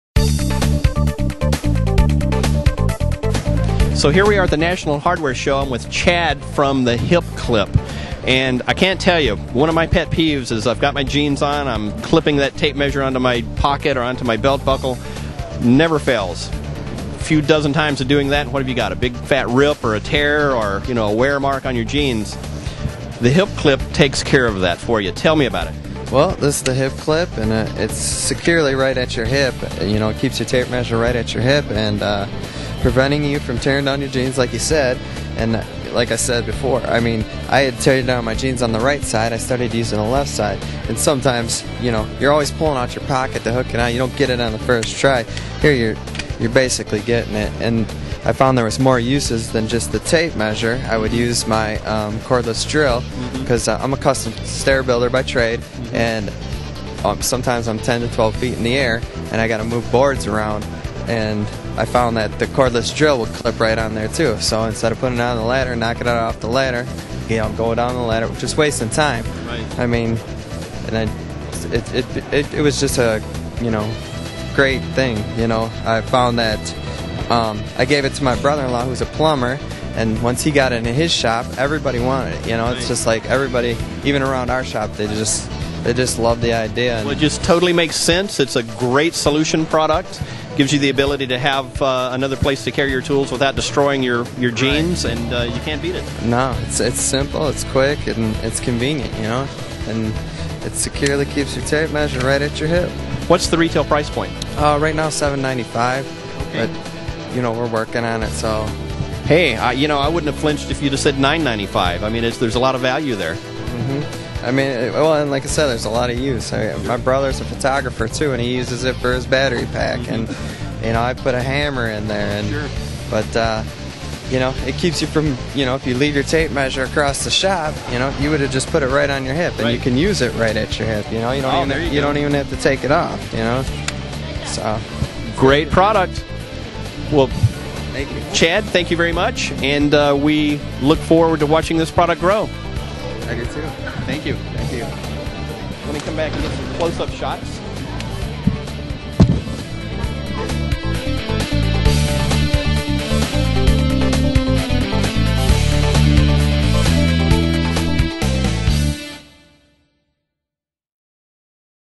Billy Carmen Talks About the Hip Clip at the National Hardware Show New Invention News
Billy Carmen Talks About the Hip Clip at the National Hardware Show New Invention News, new product launch, invention marketing